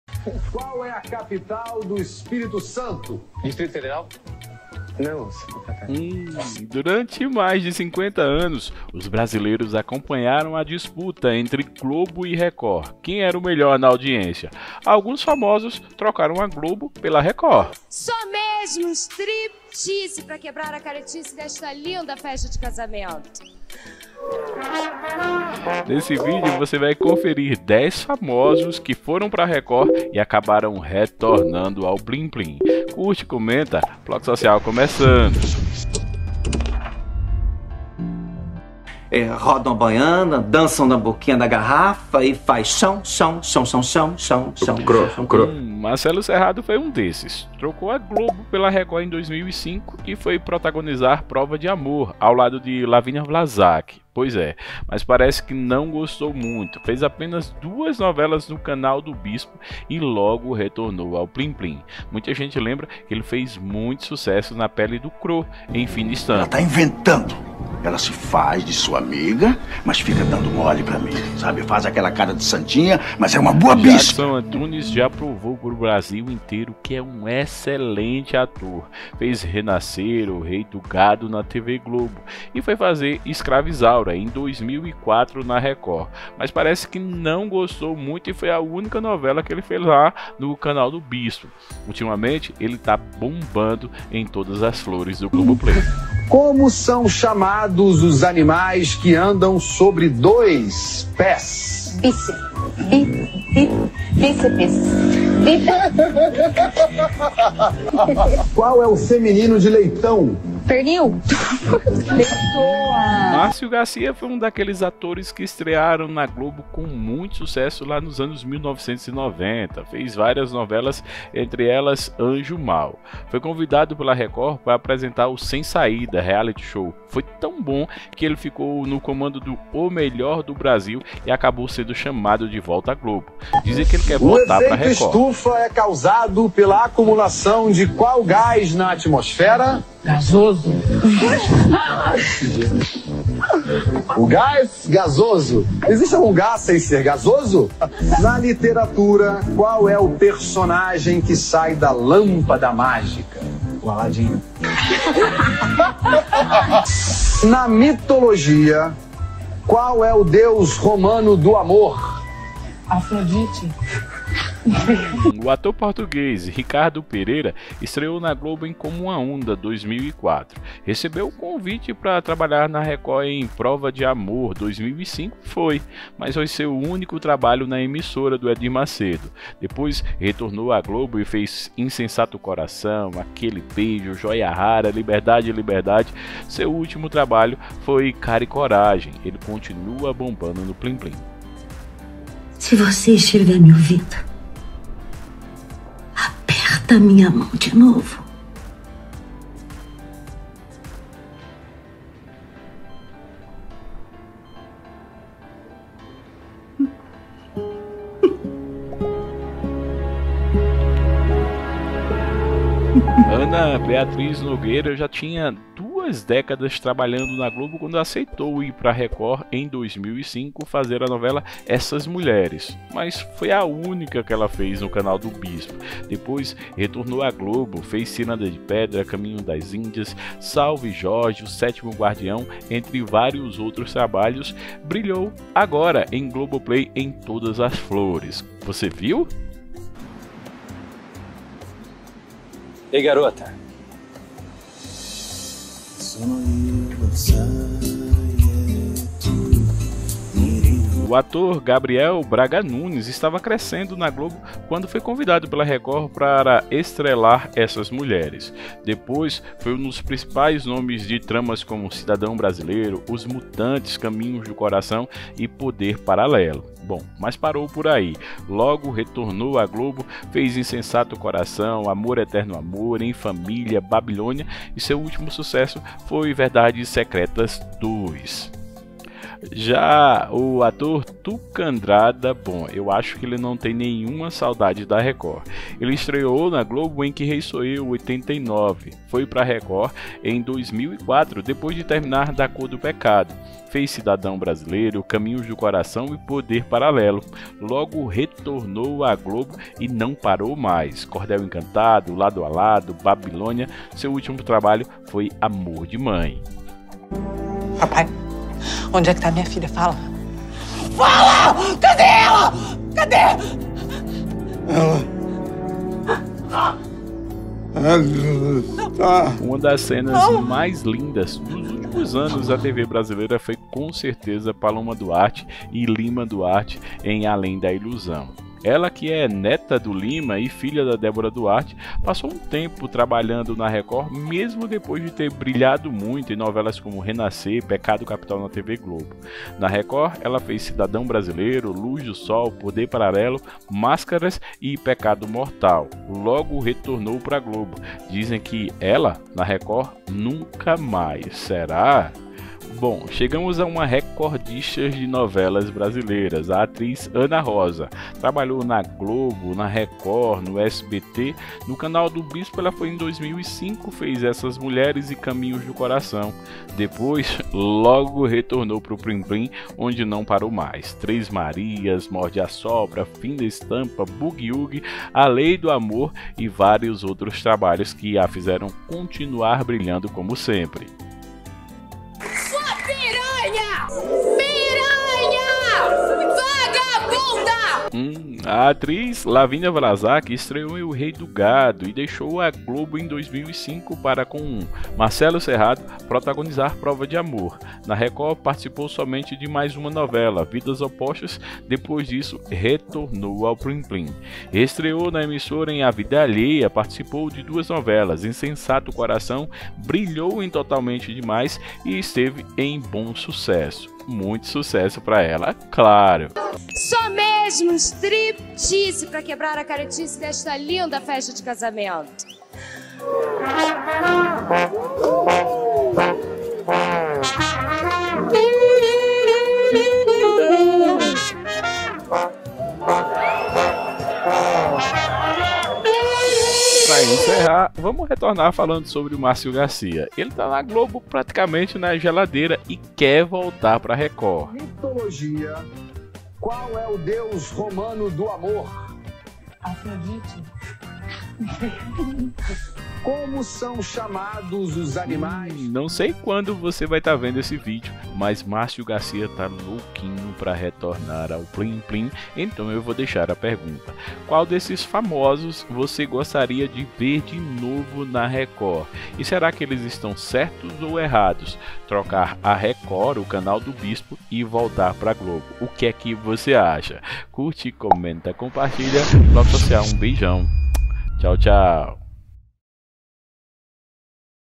10 ATORES QUE TROCARAM A GLOBO PELA RECORD, SE ARREPENDERAM E VOLTARAM AO PLIM PLIM
Veja nesse video um pouco da história de 10 atores e atrizes famosos que fizeram sucesso na Globo e acabaram indo para Record TV, o canal do Bispo Edir Macedo, mas se arrependeram e acabaram voltando ao canal da família Marinho. Entre eles a atriz de o Rei do Gado, A Lia Mezenga Lavinia Vlasak, as atrizes Ana Beatriz Nogueira, Paloma Duarte, Ana Rosa, os atores Gabriel Braga Nunes, Ricardo Pereira, Tuca Andrada, Jackson Antunes e o apresentador e ator Marcio Gárcia.

Atores famosos que trocaram a Globo pela Record e se arrependeram
- Marcelo Serrado (Crô - Fina Estampa) - 0:38
- Jackson Antunes - 1:06
- Márcio Garcia - 1:35
- Ricardo Pereira (Ator Português) - 2:45
- Ana Beatriz Nogueira (Atriz de Todas as Flores) - 3:10
- Gabriel Braga Nunes - 4:00
- Tuca Andrada - 4:40
- Paloma Duarte 5:07
- Ana Rosa - 5:52
- Lavínia Vlasak - 6:22